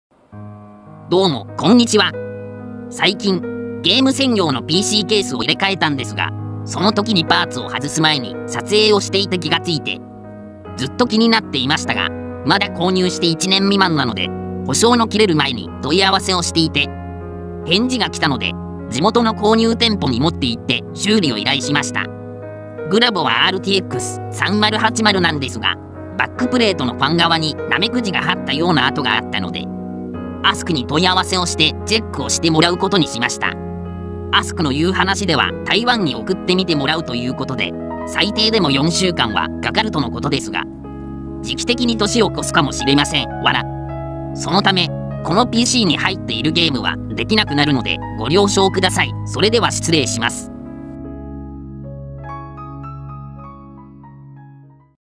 MSI GAMING X TRIO RTX3080 台湾で治療する
最近メインのゲーム専用のＰＣのケースを入れ替えた時に気が付いて問い合わせ中だったRTX3080を今回治療のために台湾まで行くことになりました。
治療の内容はヒートシンクとチップの間に貼ってあるサーマルパッドの状態が悪いのか、ゲームやベンチマークソフトを起動するとファンが止まることが無くなってケース内の温度もかなり高いので点検をしてもらうことにしました。
このPCに入っているゲームはしばらくできないようになってしまったので在庫で持っているRTX2070を付けてゲームをやってみたんですがやる気になれないくらいの差があったのでゲームはやめました。